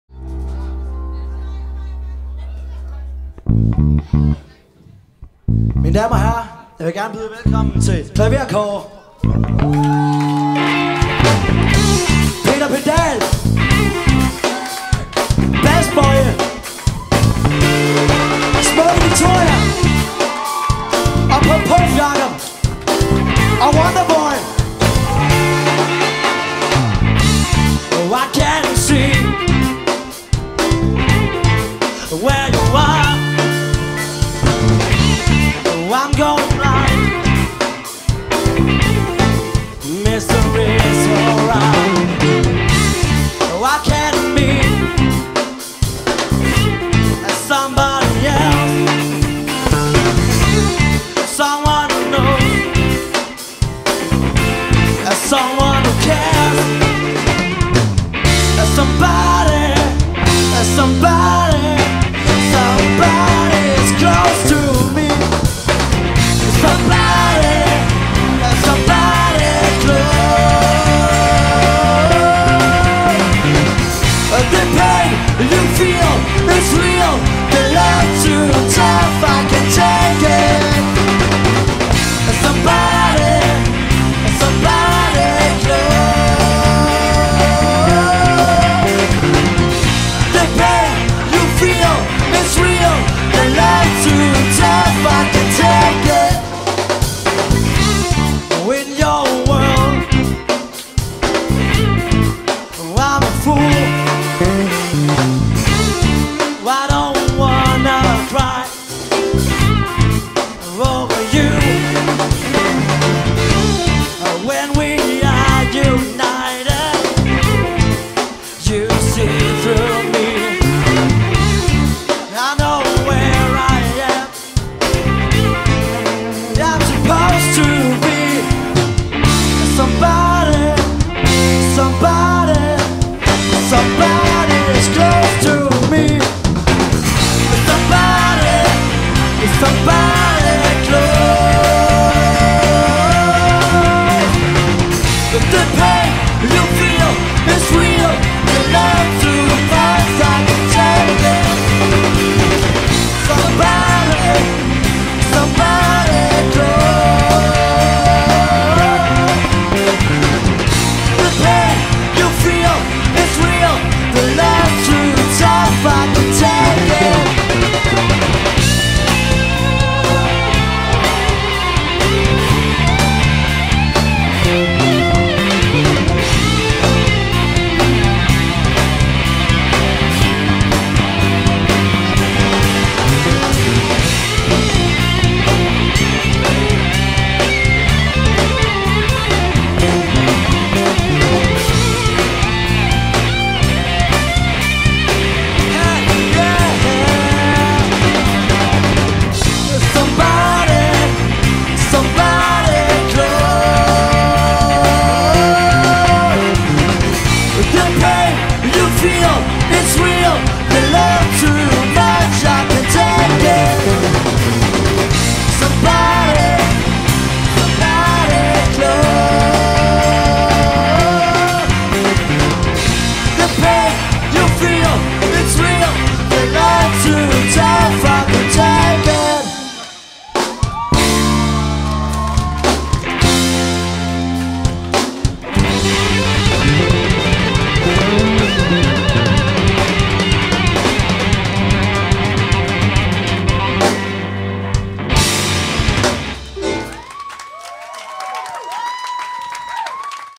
Fives - LIve på Kaosambassaden
Fives fra Mellerup Efterskole live på Kaosambassaden April 2011. Tjek for mere info.